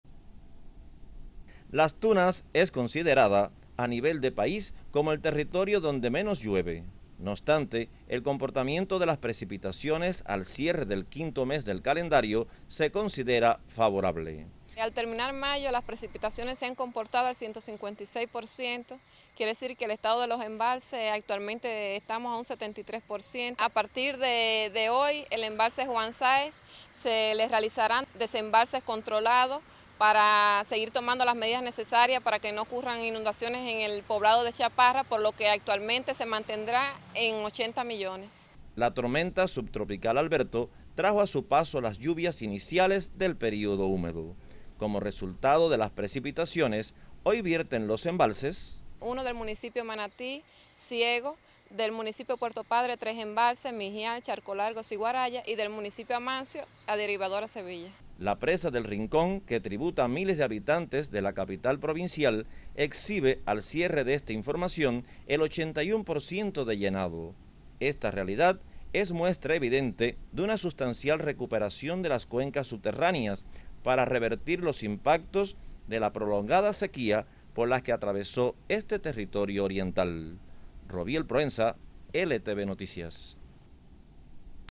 Cierra mes de mayo con favorables situación hídrica en Las Tunas
La provincia de Las Tunas cierra el mes de mayo con un 73 % de llenado en los embalses. En el territorio alivian 5 presas y otras están en prevención hídrica